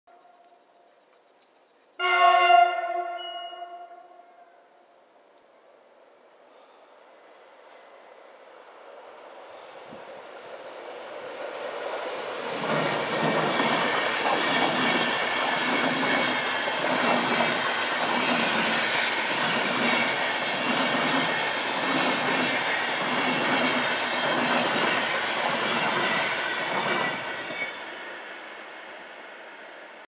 InterCity bei Estorf
Eine 101 fährt mit ihrem IC 2320 (Frankfurt-Hamburg) am Einfahrsignal des Bhf. Estorf vorbei - der Zug wurde wegen Bauarbeiten bei Bremen ausnahmsweise hier langgeleitet. Aufnahme vom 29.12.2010